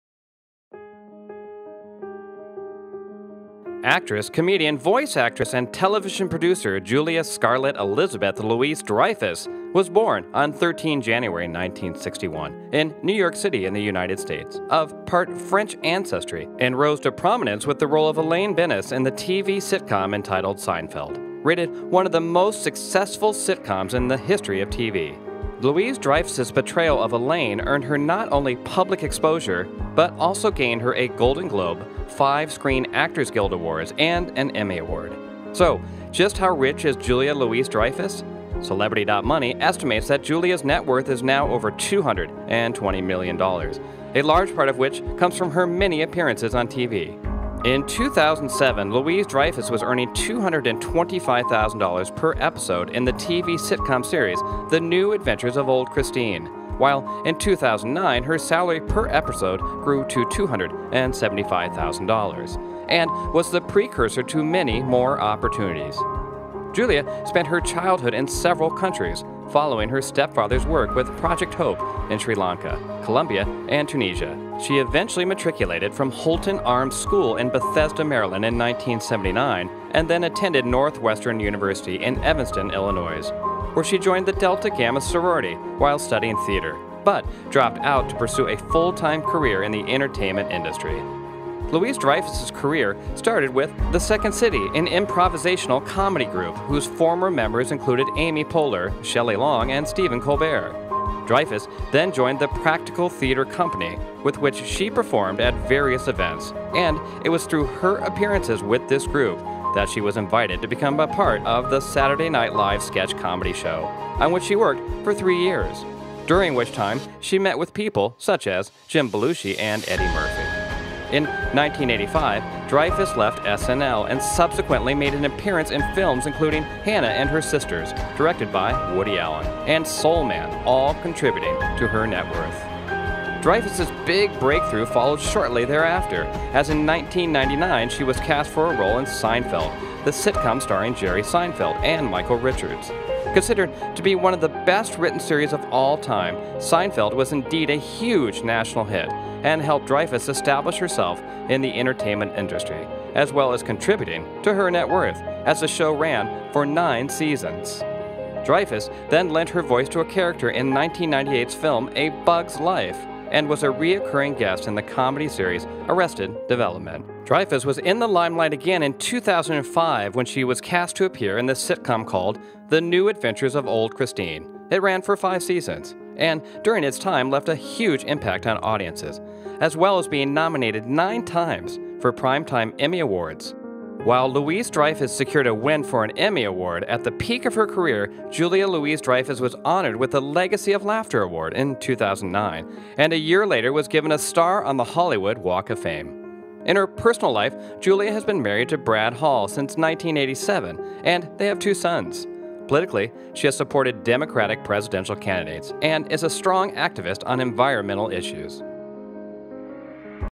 Julia Louis-Dreyfus: Short Biography, Net Worth & Career Highlights
Actress, comedian, voice actress and television producer Julia Scarlett Elizabeth Louis-Dreyfus was born on 13 January 1961, in New York City, USA, of part-French ancestry, and rose to prominence with the role of Elaine Benes in the TV sitcom entitled “Seinfeld”, rated one of the most successful sitcoms in the history of TV. Louis-Dreyfus’ portrayal of Benes earned her not only public exposure, but also gained her a Golden Globe, five Screen Actors Guild, and an Emmy Award.
So just how rich is Julia Louis-Dreyfus? Celebrity dot money estimates that Julia’s net worth is now over 220 million dollars, a large part of which comes from her many appearances on TV. In 2007 Louis-Dreyfus was earning $225,000 per episode in the TV sitcom series “The New Adventures of Old Christine”, while in 2009 her salary per episode grew to $275,000, and was the precursor to many more opportunities.
Julia spent her childhood in several countries, following her stepfather’s work with Project HOPE in Sri Lanka, Colombia and Tunisia, amongst others. She eventually matriculated from Holton-Arms School in Bethesda, Maryland in 1979, and then attended Northwestern University in Evanston, Illinois, where she joined the Delta Gamma sorority while studying theatre, but dropped out to pursue a full-time career in the entertainment industry.[
Louis-Dreyfus’ career started with “The Second City”, an improvisational comedy group, whose former members included Amy Poehler, Shelley Long and Stephen Colbert. Dreyfus then joined the “Practical Theatre Company” with which she performed at various events, and it was through her appearances with this group that she was invited to become a part of the “Saturday Night Live” sketch comedy show, on which she worked for three years, during which time she met people such as Jim Belushi and Eddie Murphy. In 1985, Dreyfus left SNL and subsequently made an appearance in films including “Hannah and Her Sisters” directed by Woody Allen, and “Soul Man”, all contributing to her net worth.
Dreyfus’ big breakthrough followed shortly after, as in 1990 she was cast for a role in “Seinfeld”, the sitcom starring Jerry Seinfeld and Michael Richards. Considered to be one of the best written series of all time, “Seinfeld” was indeed a huge national hit, and helped Dreyfus establish herself in the entertainment industry, as well as contributing to her net worth as the show ran for nine seasons.
Dreyfus then lent her voice to a character in the 1998 film “A Bug’s Life”, and was a recurring guest in the comedy series “Arrested Development”. Dreyfus was in a limelight again in 2005 when she was cast to appear in the sitcom called “The New Adventures of Old Christine”; it ran for five seasons, and during its time left a huge impact on audiences, as well as being nominated nine times for Primetime Emmy Awards, while Louis-Dreyfus secured a win for an Emmy Award. At the peak of her career, Julia Louis-Dreyfus was honored with the Legacy of Laughter award in 2009, and a year later was given a star on the Hollywood Walk of Fame.
In her personal life, Julia has been married to Brad Hall since 1987, and they have two sons. Politically, she has supported Democratic presidential candidates, and is a strong activist on environmental issues.